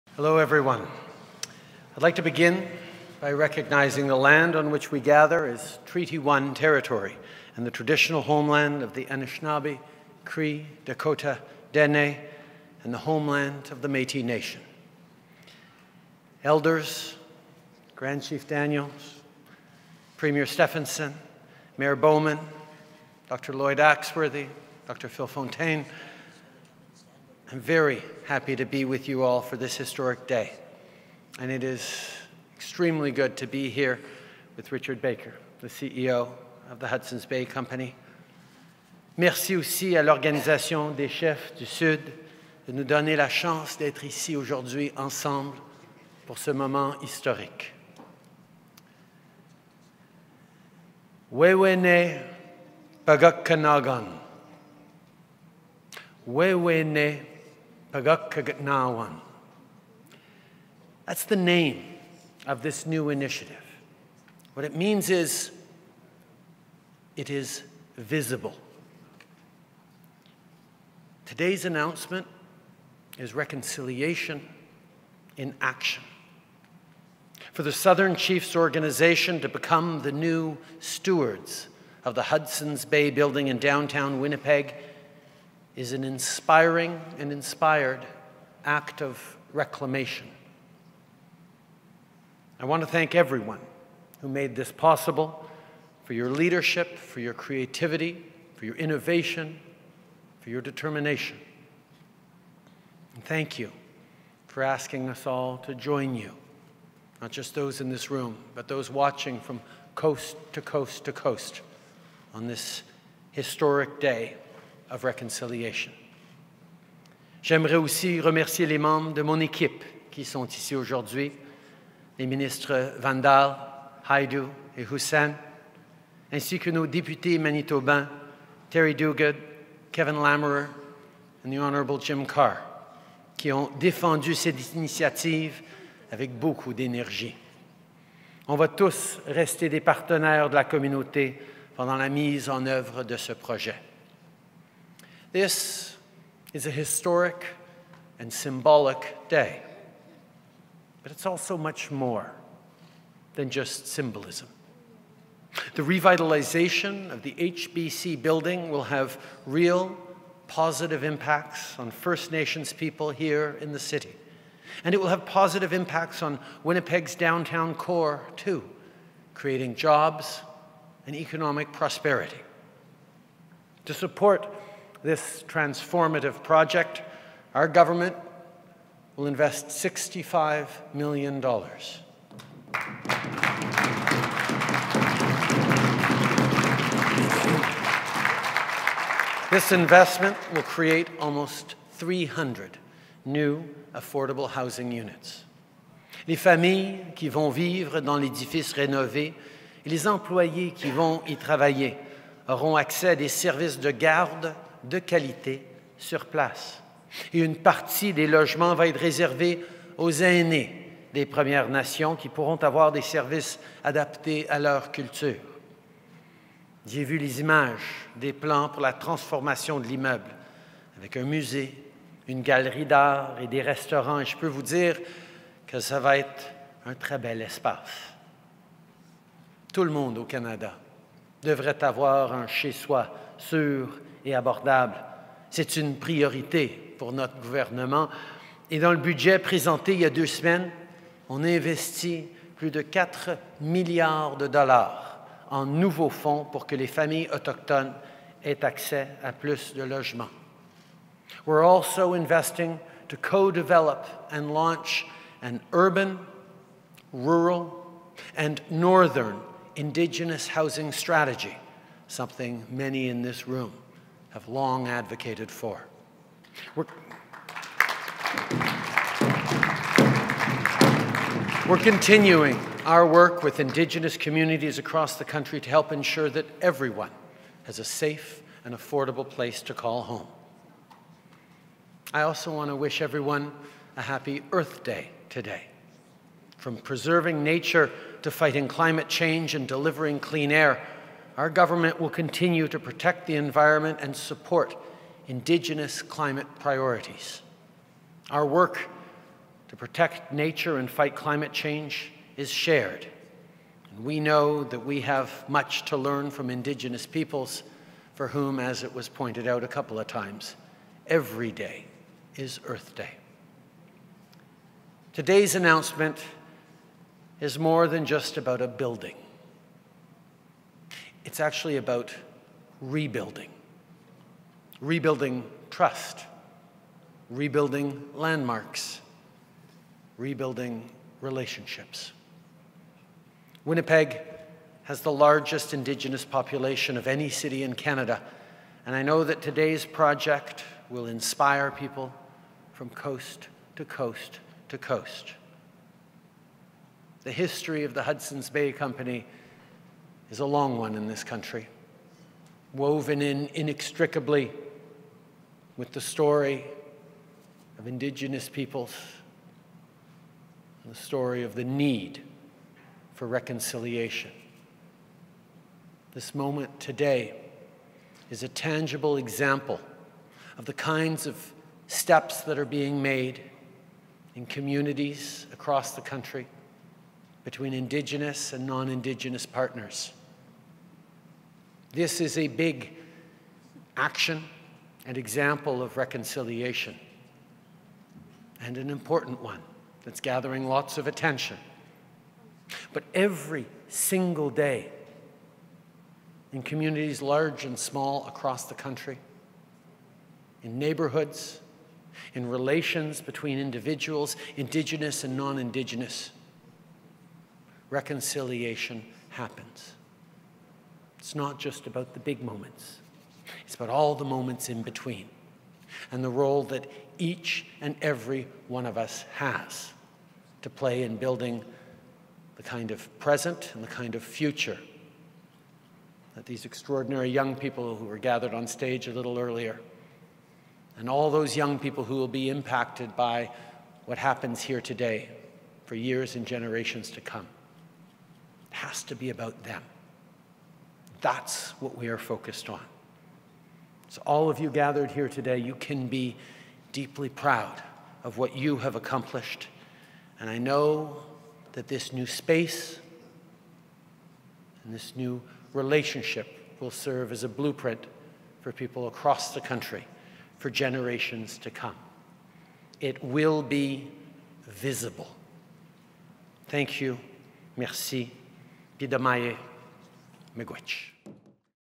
Offrir des logements abordables aux peuples autochtones de Winnipeg
22 avril 2022
Le premier ministre Justin Trudeau annonce un nouvel investissement fédéral qui permettra d’offrir près de 300 nouveaux logements abordables à des membres des Premières Nations de la région, y compris des familles, des Aînés et des étudiants postsecondaires.